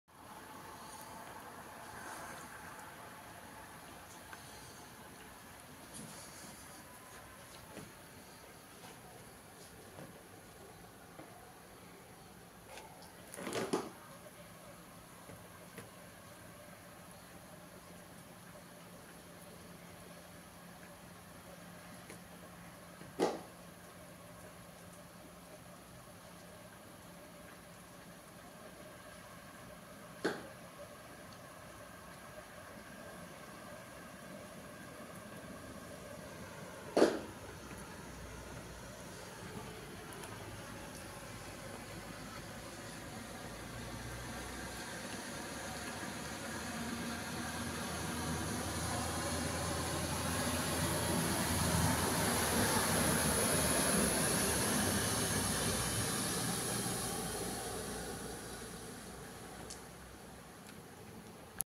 Maltempo, Torrette di nuovo allagata nella notte
Ancora pioggia, ancora allagamenti a Fano sud, tra cui Torrette. Secondo il sindaco Luca Serfilippi gli interventi non sono più rimandabili.

LEGGI LA NOTIZIA: